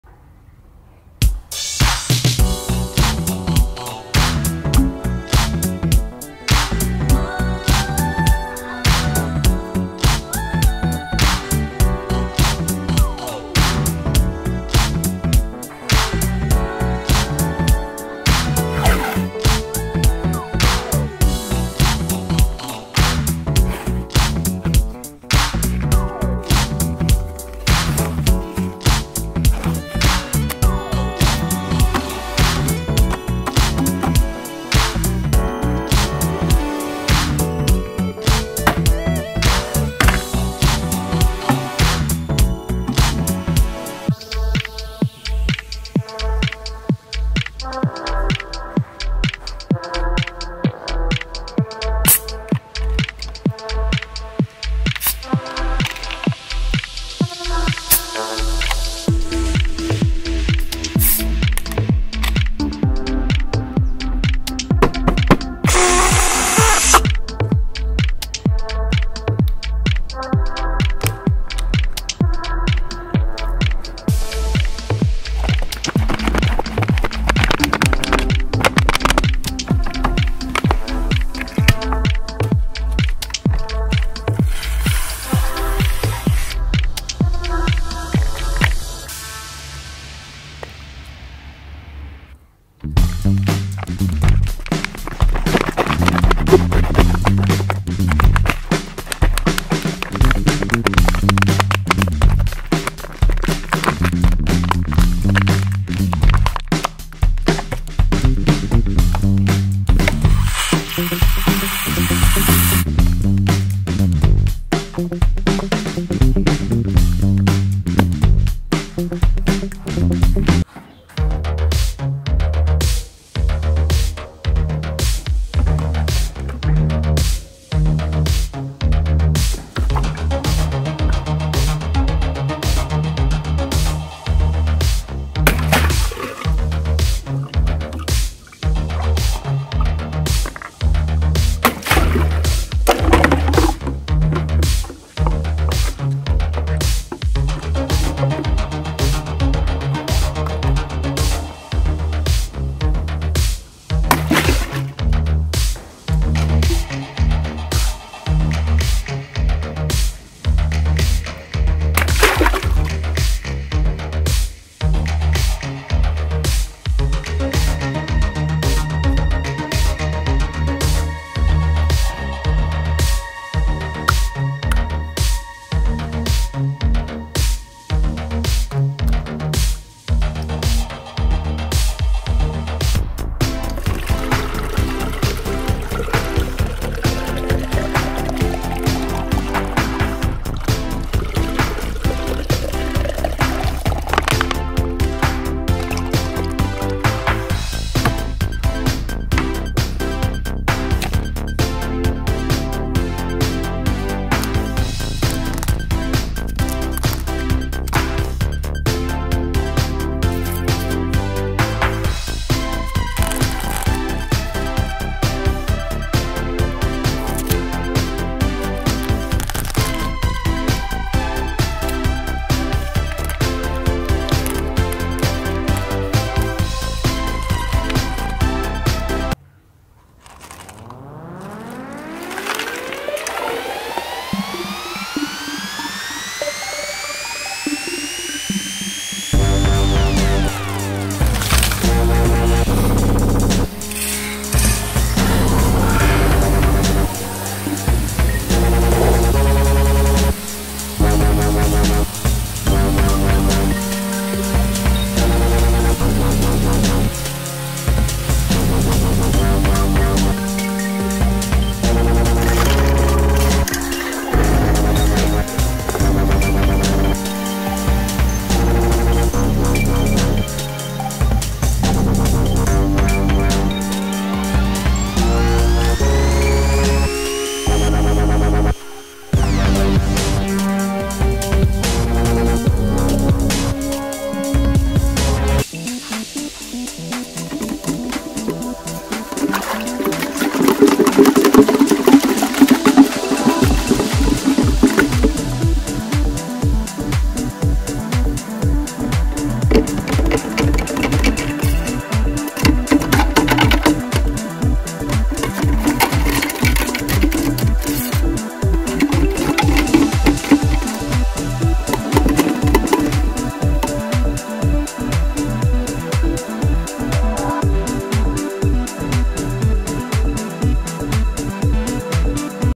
Experiment !! BALLOONS vs Cola, Sprite, Fanta, Pepsi and Mentos in toilet
Experiment video : BALLOONS vs Cola, Sprite, Fanta, Pepsi and Mentos in toilet